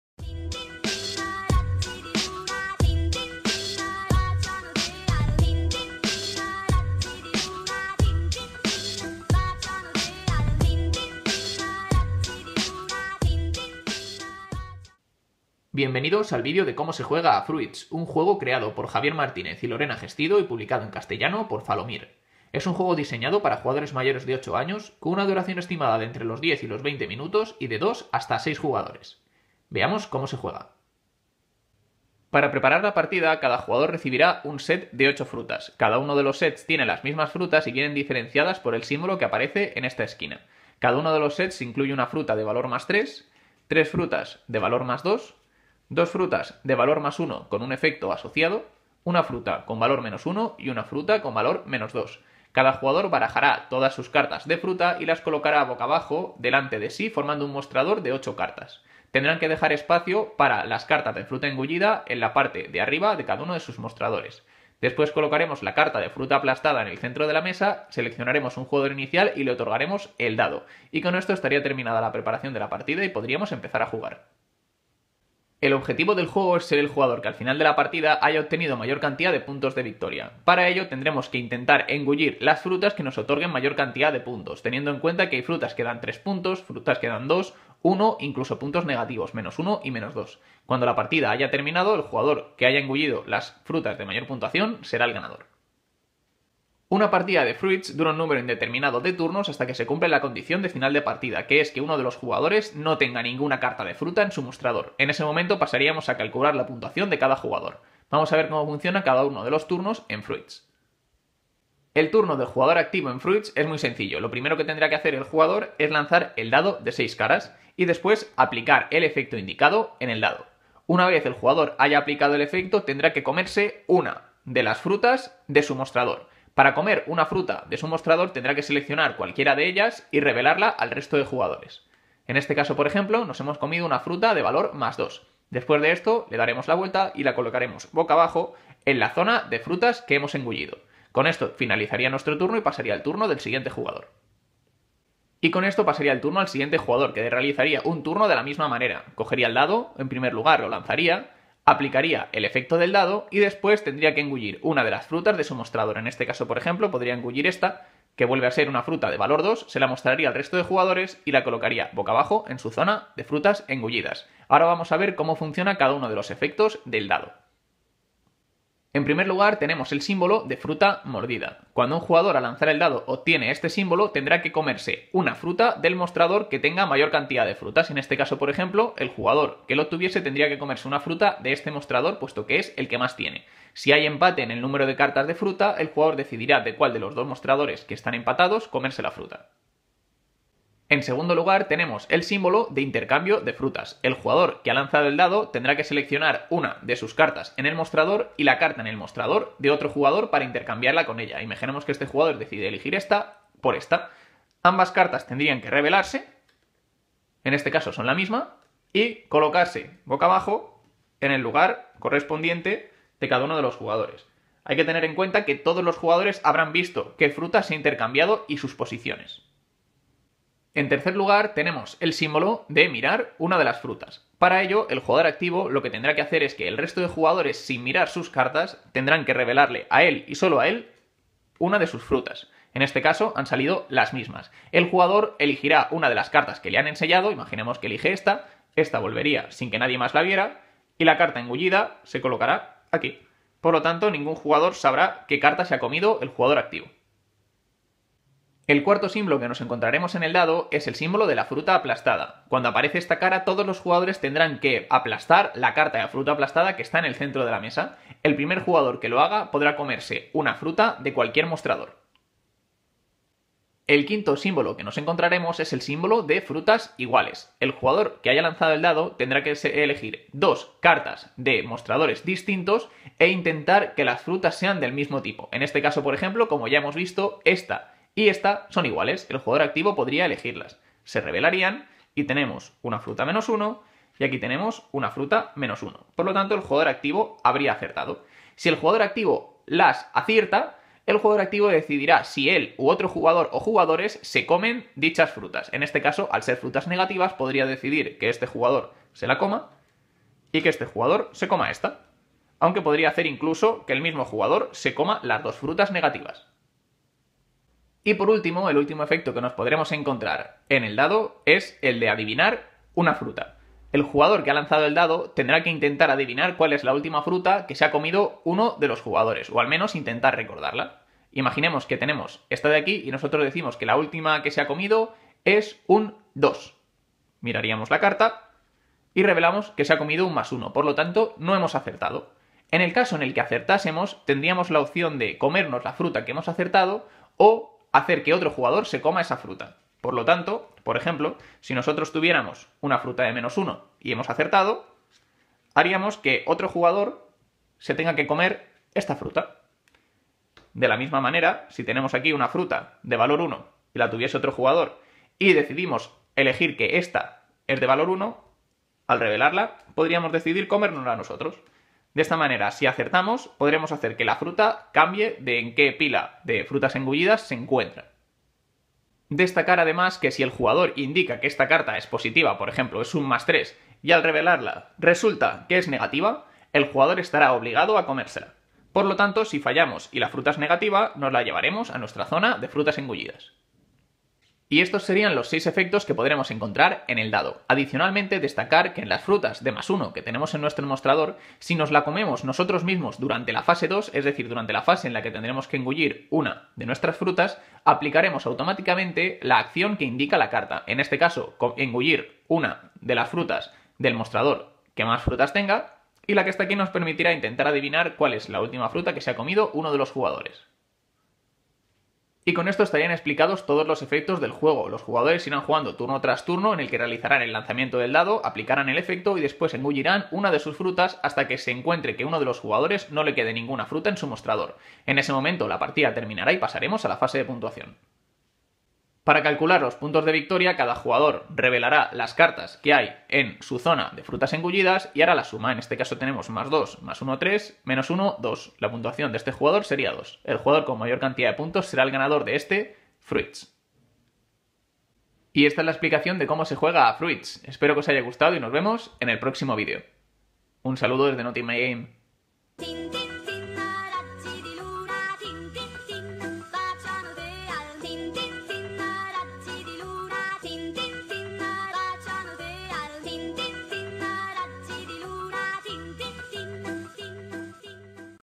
Fruits | Cómo se juega
Puedes comprar tus juegos con un % de descuento adicional en LOS JUEGOS DE SAURON con el código NOTINMYGAME

Explicación detallada de cómo se juega a Fruits, un juego diseñado por Javier Martínez y Lorena Gestido, y publicado en castellano por Falomir.